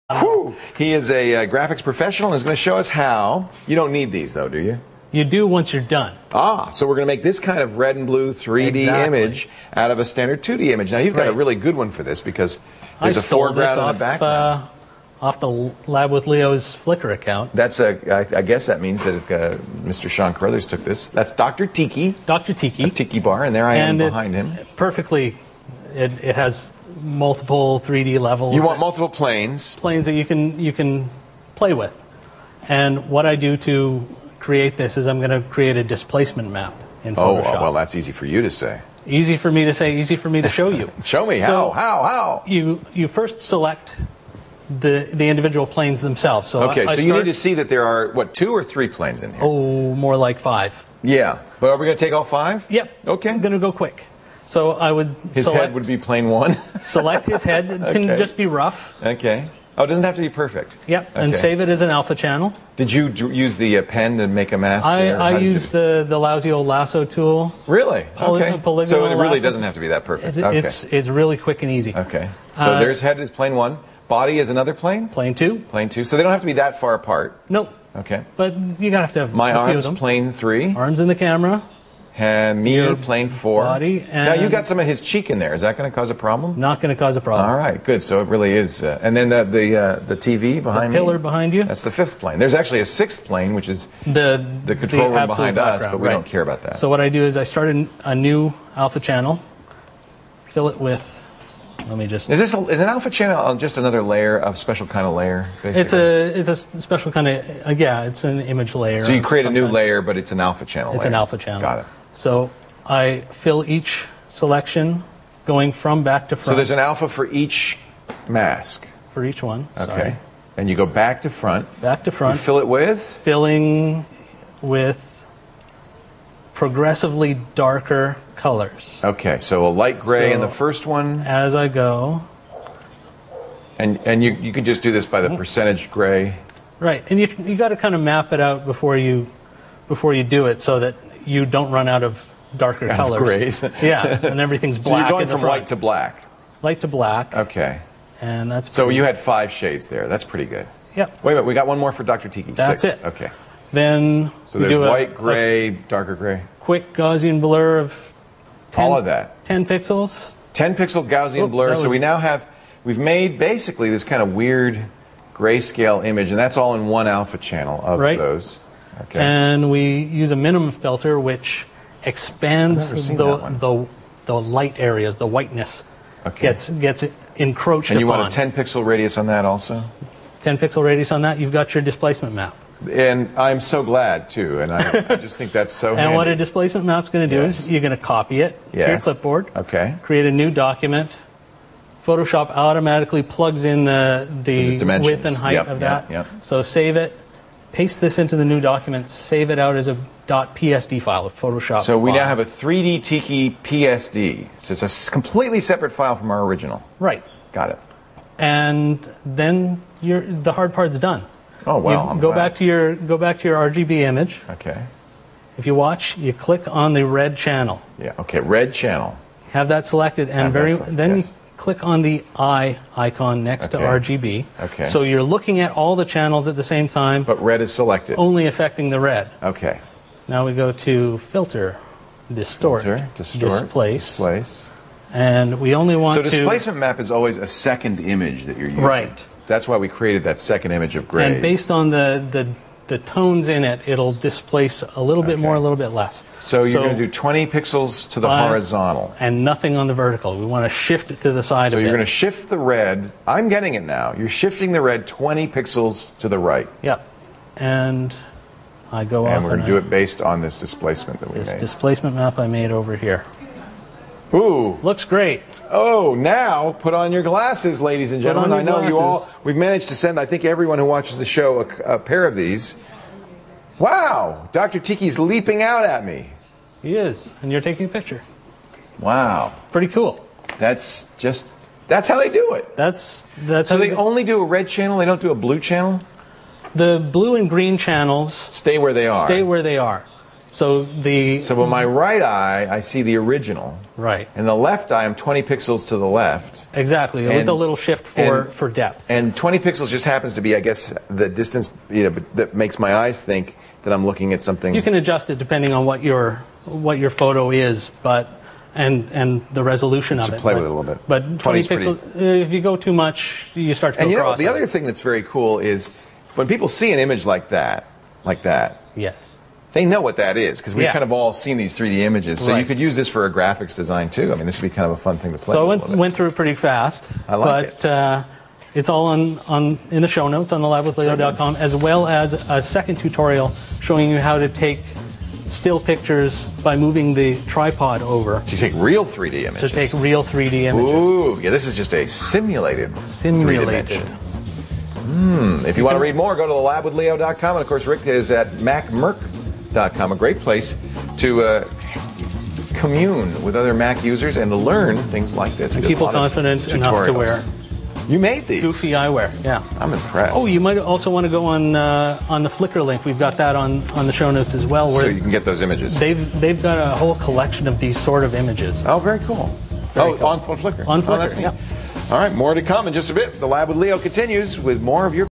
make rgb 3d pictures in photoshop
leo laporte learns how to make a rgb 3d image in photoshop. also a link to a tutorial on how to make a real 3d image at the end of the clip